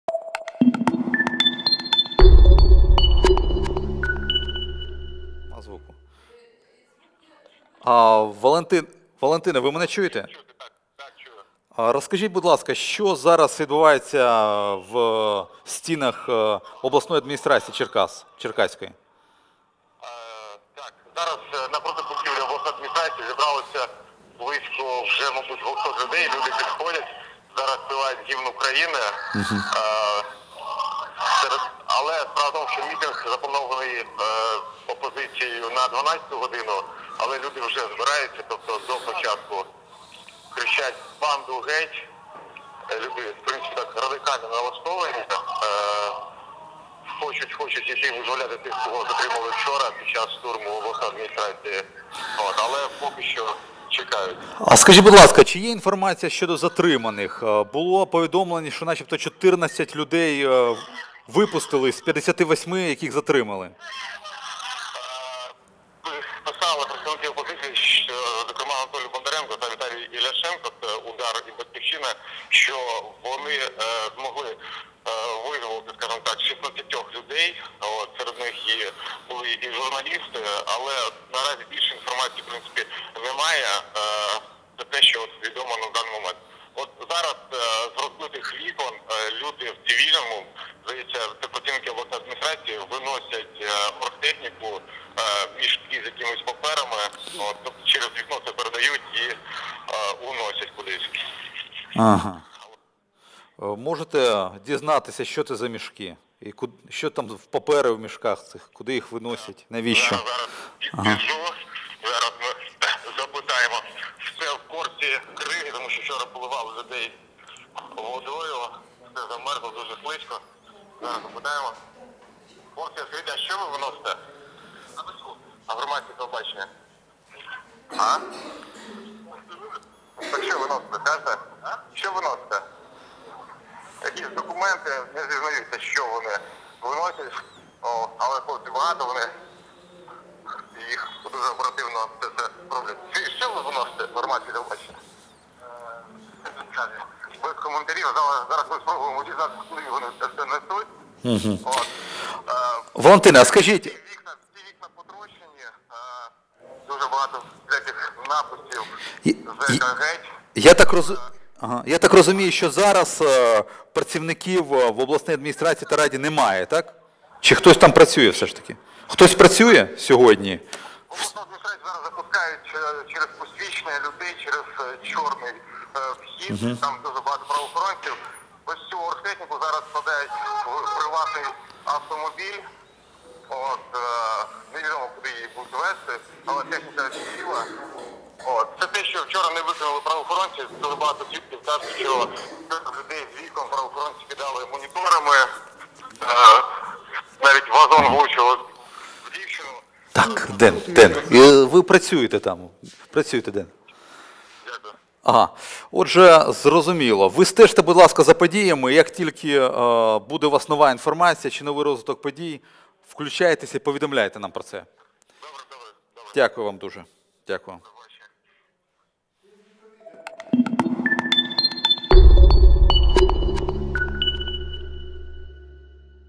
Черкаська ОДА в облозі
Журналіст Валентин Чернявський про події в Черкасах телефоном на Hromadske.TV 24.01.14